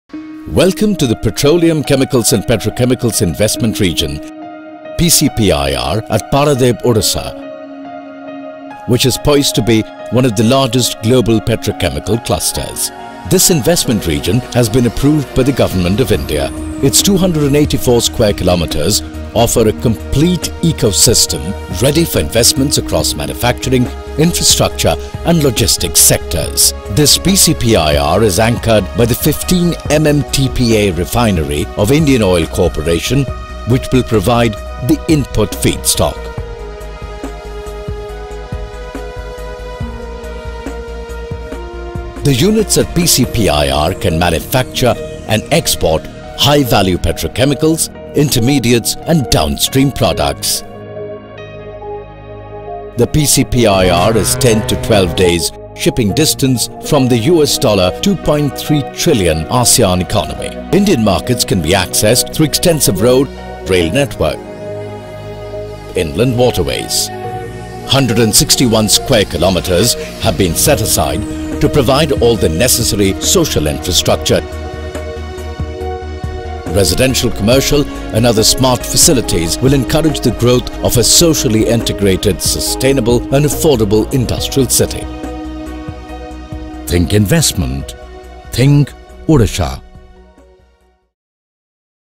Feedback's Master Planning work on PCPIR in Odisha
Feedback's Master Planning work on the Petroleum, Chemicals and Petrochemicals Investment Region in Odisha.